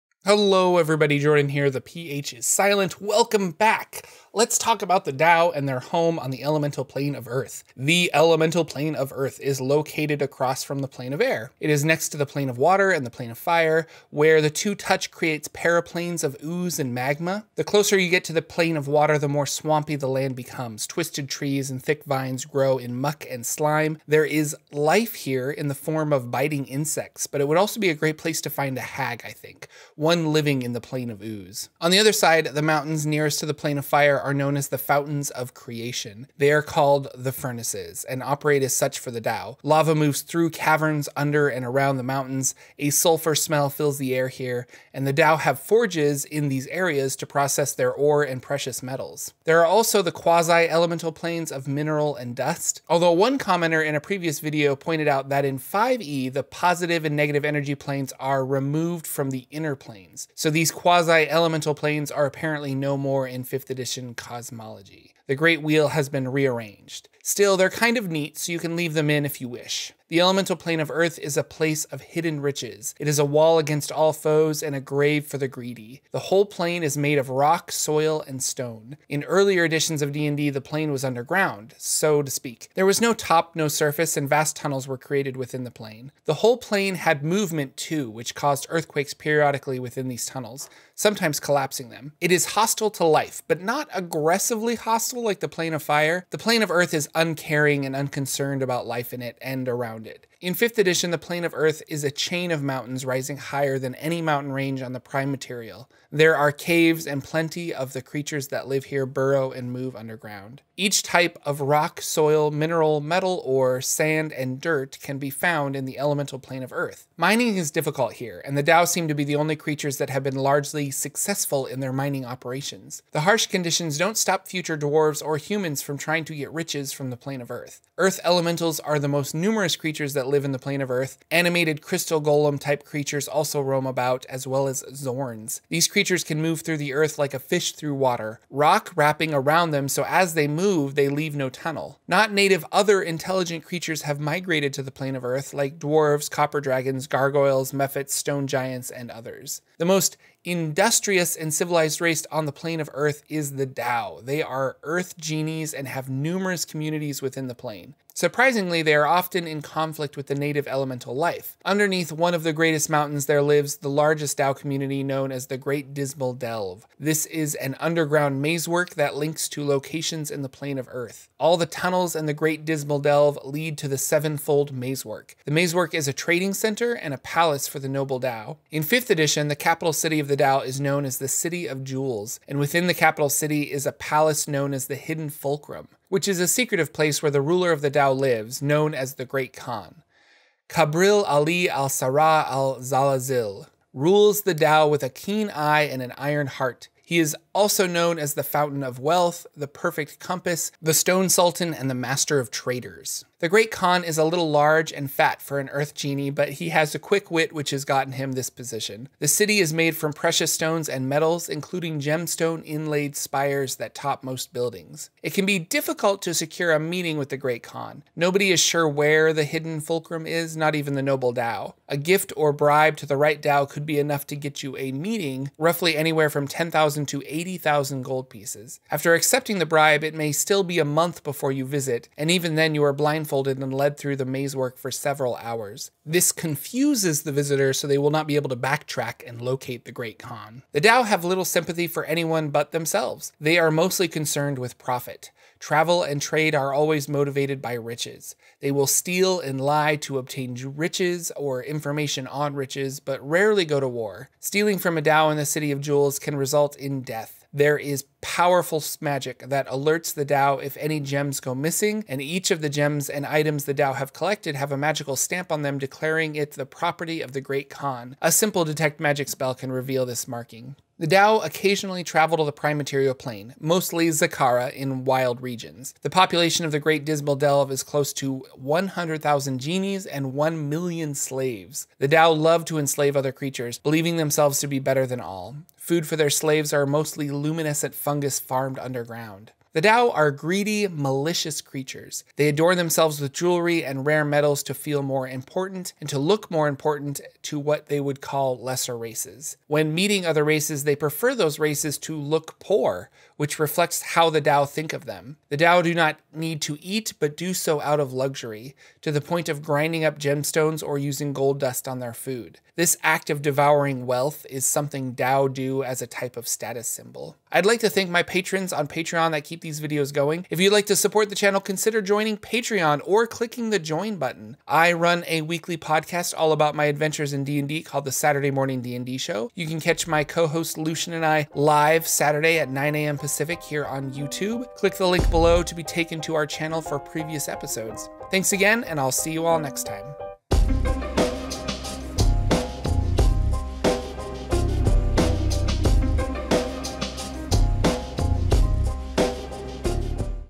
D&D Lore - The Dao and the City of Jewels
Episode 120! In this episode we talk about the Dao and the City of Jewels located on the Elemental Plane of Earth!

👕 Forgotten Realms Explained T-Shirts!

Saturday Morning D&D Show!

Or check it out in Podcast Format! 🎙️

References:
Dungeon Master’s Guide 5e
Monster Manual 5e